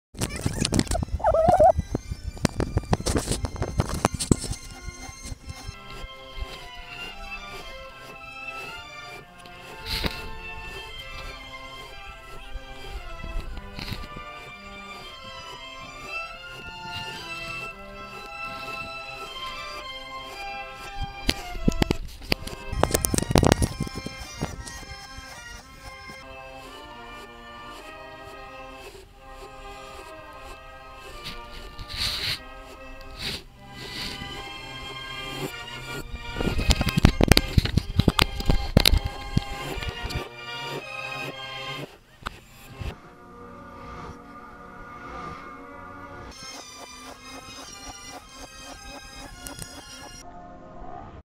reversememory #5 I remember the Santiago procession in Madrid
I was walking in Madrid until we saw the Santiago procession in Plaza Santiago. It was very random. There was a speech and free sangria afterwards.